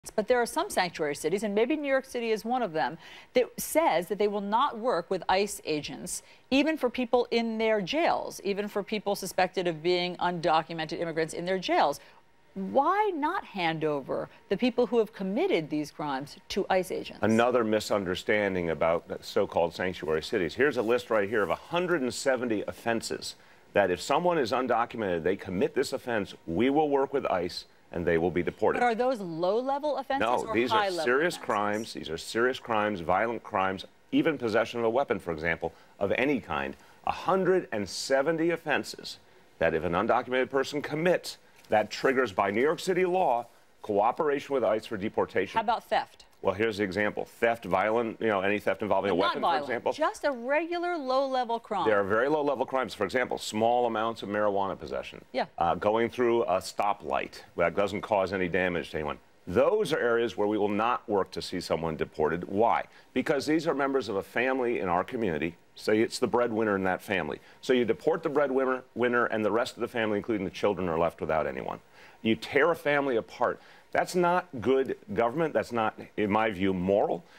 Dodgy De Blasio Seems to say NYC Won't Help Deport Illegal Immigrant Thieves
On CNN, NYC Mayor Bill de Blasio seems to indicate that the city will not cooperate with federal immigration authorities to deport non-violent thieves, on the theory that they might be their families' "breadwinner."